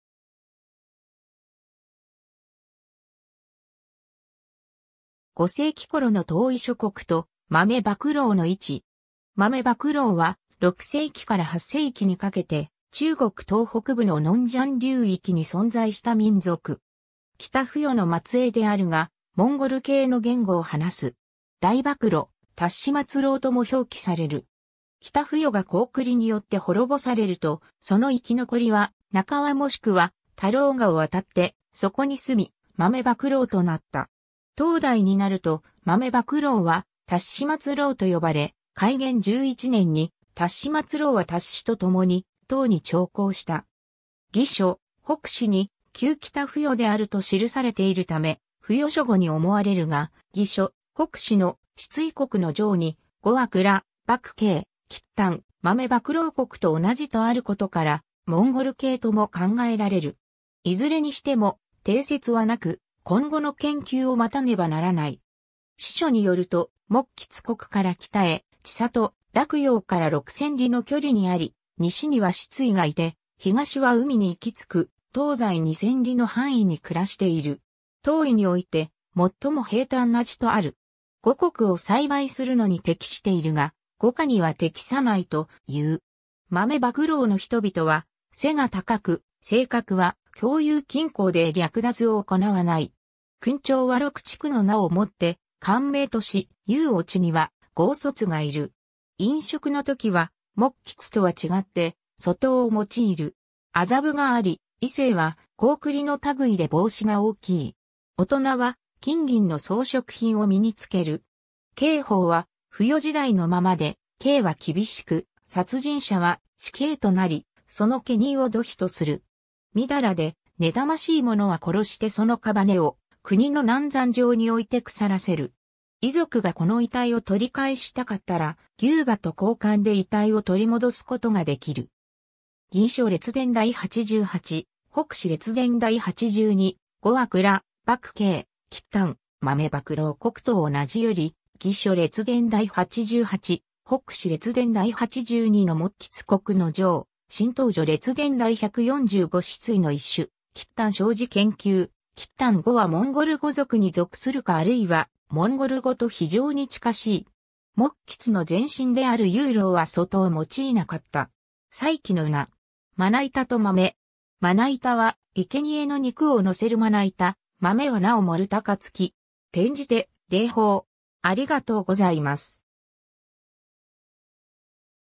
豆莫婁
5世紀頃の東夷諸国と豆莫婁の位置。
 豆莫婁（呉音：づまくる、漢音：とうばくろう、拼音：Dòumòlóu）は、6世紀から8世紀にかけて中国東北部の嫩江流域に存在した民族。
北夫余の末裔であるが、モンゴル系の言語を話す。
大莫盧・達末婁とも表記される。
 北夫余が高句麗によって滅ぼされると、その生き残りは那河もしくは他漏河を渡ってそこに住み、豆莫婁となった。
 唐代になると豆莫婁は達末婁と呼ばれ、開元11年（723年）に達末婁は達姤とともに唐に朝貢した。
 『魏書』『北史』に「旧北扶余である」と記されているため、扶余諸語に思われるが、『魏書』『北史』の失韋国の条に「語は庫莫奚・契丹・豆莫婁国と同じ」とあることから、モンゴル系とも考えられる。
いずれにしても、定説はなく、今後の研究を待たねばならない。
 史書によると、勿吉国から北へ千里、洛陽から6千里の距離に在り、西には室韋がいて東は海（日本海）に行きつく、東西2千里の範囲に暮らしている。
「東夷において最も平坦な地」とある。
五穀を栽培するのに適しているが、五果（桃・李・杏・棗・栗）には適さないという。
 豆莫婁の人々は背が高く、性格は強勇謹厚で略奪を行わない。
君長は六畜（馬・牛・羊・犬・豕・鶏）の名をもって官名とし、邑落には豪帥がいる。
飲食のときは勿吉とは違って俎豆（そとう）を用いる。
麻布があり、衣製は高句麗の類で帽子が大きい。
大人は金銀の装飾品を身につける。
 刑法は夫余時代のままで、刑は厳しく、殺人者は死刑となり、その家人を奴婢とする。
淫らで妬ましい者は殺してその屍を国の南山上に置いて腐らせる。
遺族がこの遺体を取り返したかったら、牛馬と交換で遺体を取り戻すことができる。
 ^ 『魏書』列伝第八十八、『北史』列伝第八十二「語は庫莫奚・契丹・豆莫婁国と同じ」より ^ 『魏書』列伝第八十八、『北史』列伝第八十二の勿吉国の条 ^ 『新唐書』列伝第一百四十五 ^ 室韋の一種 ^ ^ 『契丹小字研究』「契丹語はモンゴル語族に属するか或はモンゴル語と非常に近しい。」 ^ 勿吉の前身である挹婁（ゆうろう）は俎豆（食器）を用いなかった。
 ^ 祭器の名。
俎と豆。
俎はいけにえの肉をのせるまないた、豆は菜を盛るたかつき。
転じて、礼法。